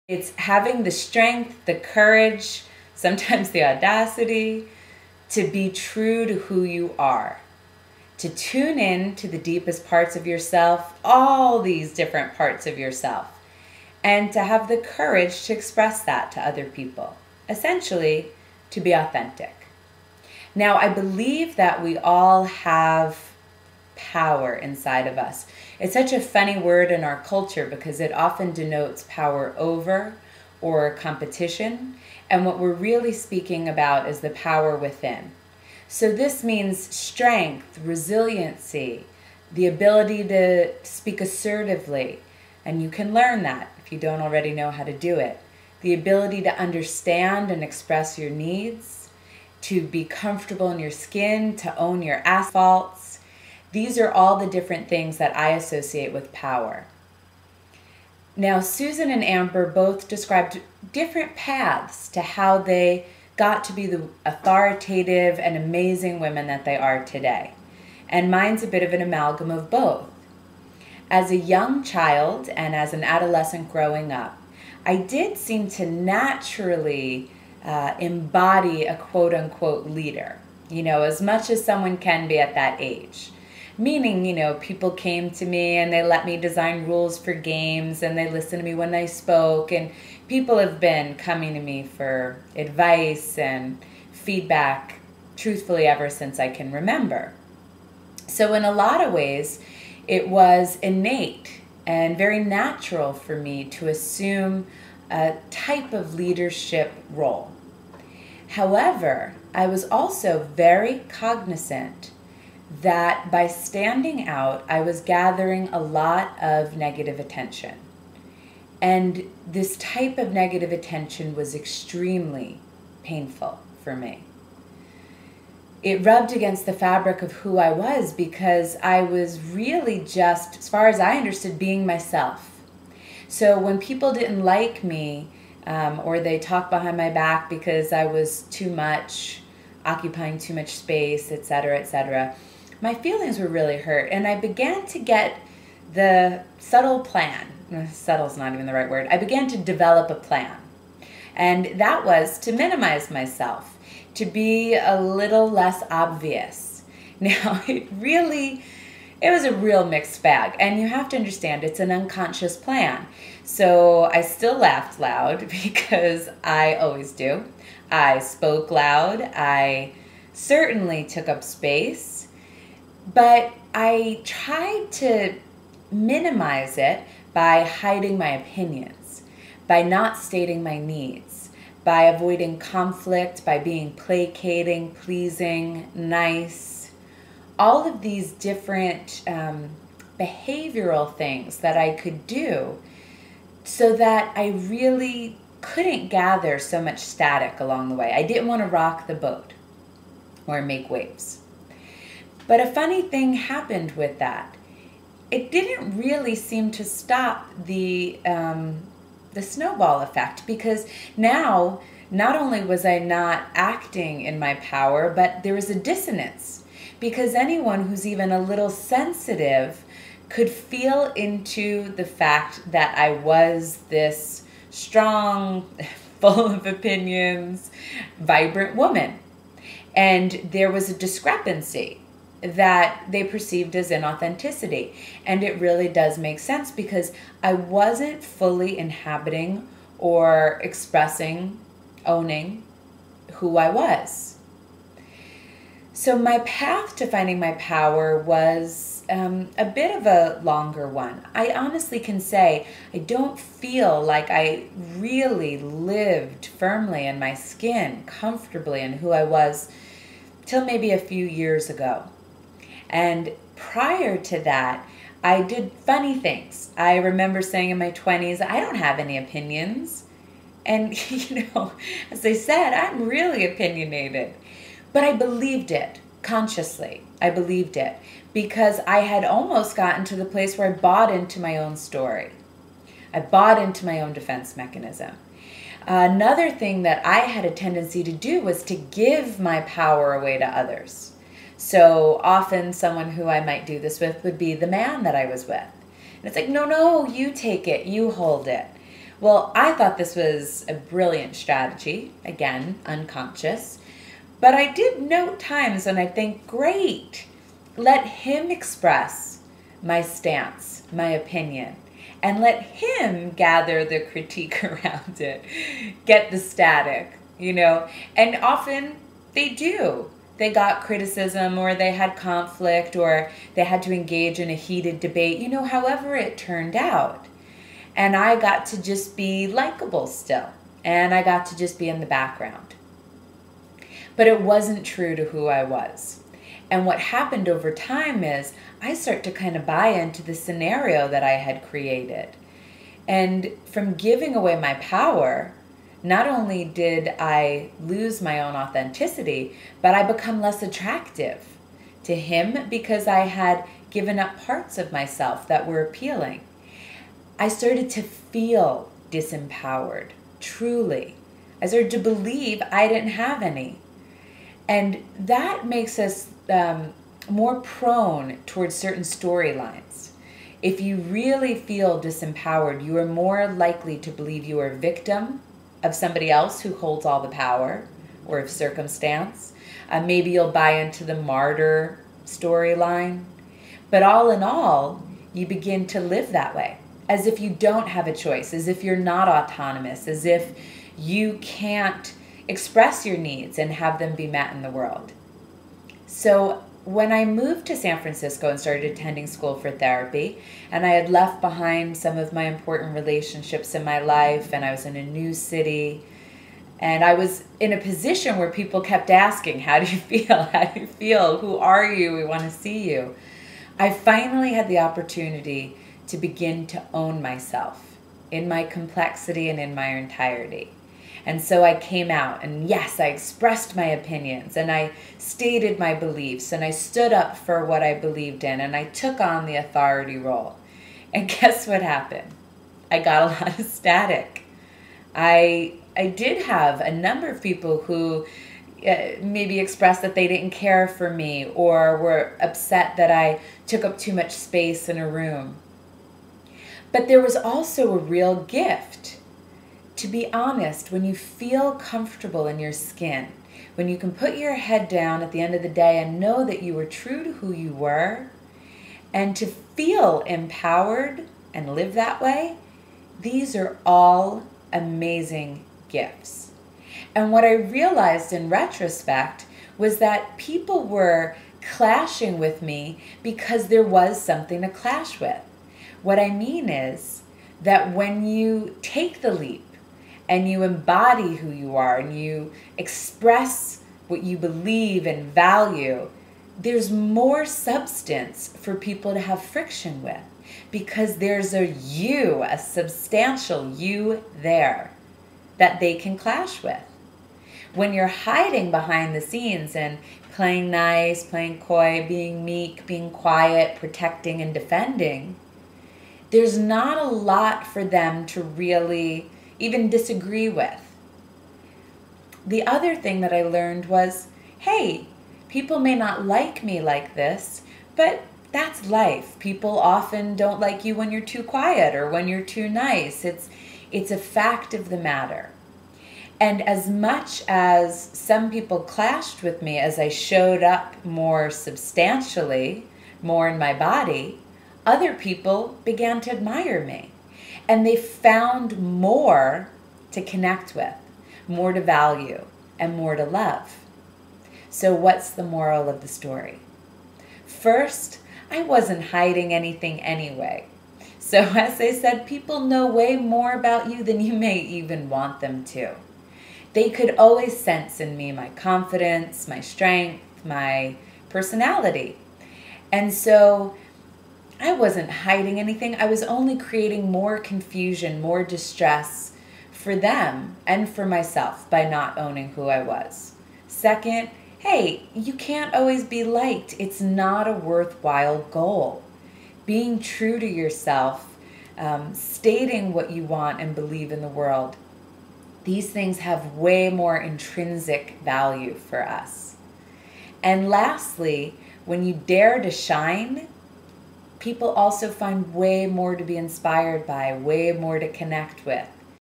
What is it to be an empowered woman.......
The Map2self after show addressing issues around women's leadership and living our power. Vanessa Scotto shares her own personal journey with embodying her power and standing in her assertiveness.

Note: Sorry my video is cut off! It was too long. Still learning how to work my camera.

To hear the show on Map2self go and listen to the 11/10/10 archive.